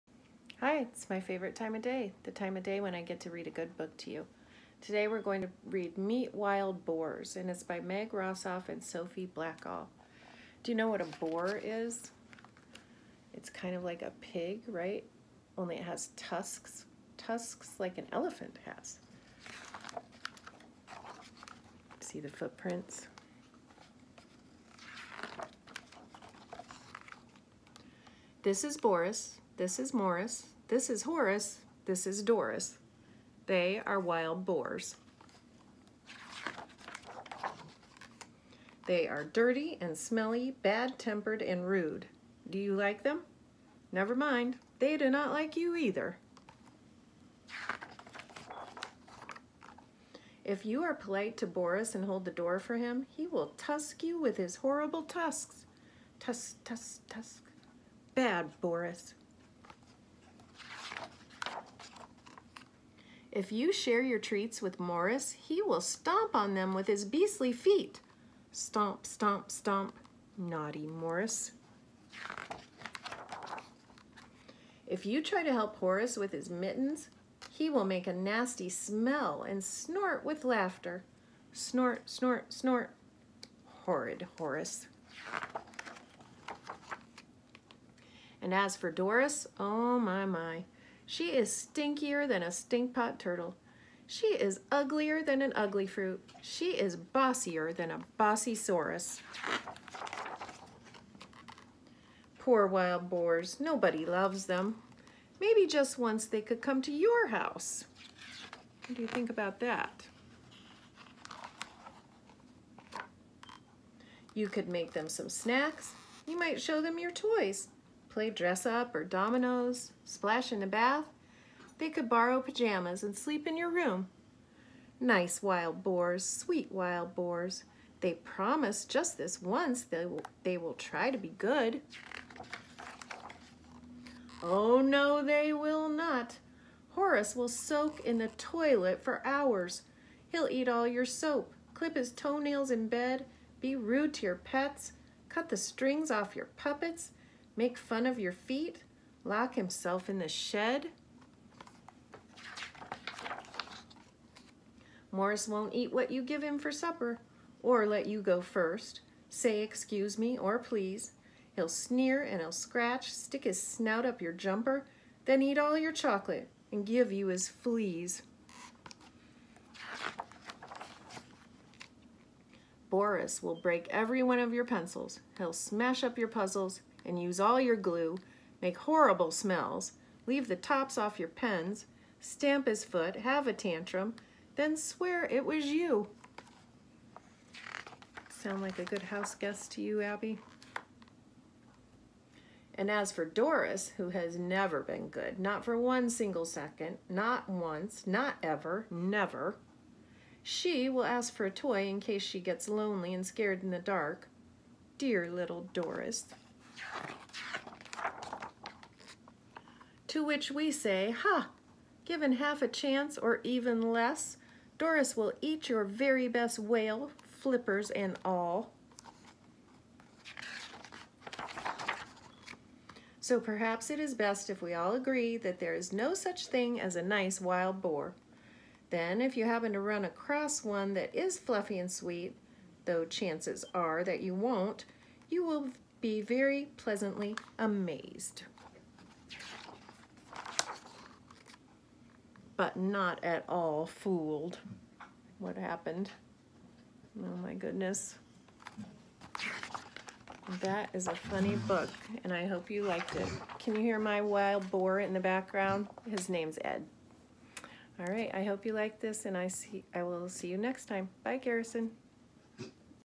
Meet Wild Boars
This is a read aloud of Meet Wild Boars by Meg Rosoff and Sophie Blackall and published by Macmillan. The AR level is 3.0 and the quiz number is 87584. I you liked this you may also like The Donkey Egg.